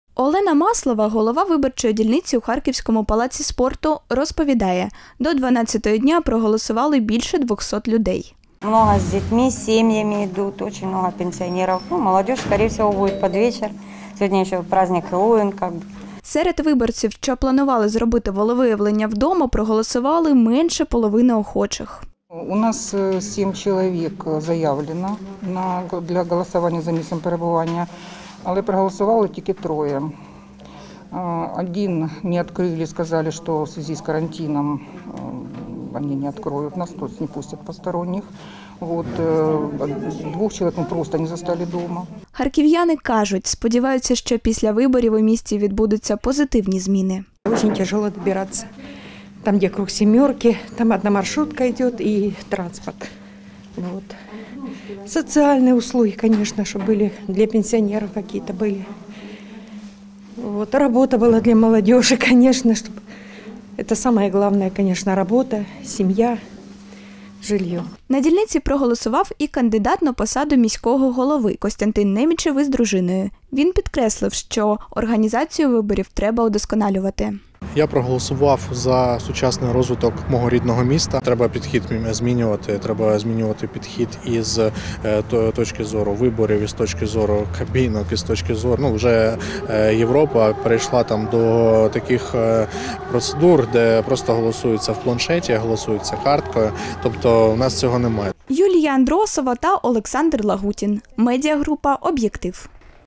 Кандидат на посаду мера Харкова Костянтин Немічев голосував у Палаці спорту
Ще один із кандидатів на посаду міського голови зробив своє волевиявлення на дільниці у Харківському Палаці спорту. Він заявив, що процедуру голосування в Україні потрібно осучаснити.

Медіагрупа "Об'єктив" - це об'єктивні новини Харківщини,
України та світу щодня у будні на телеканалі "Simon"!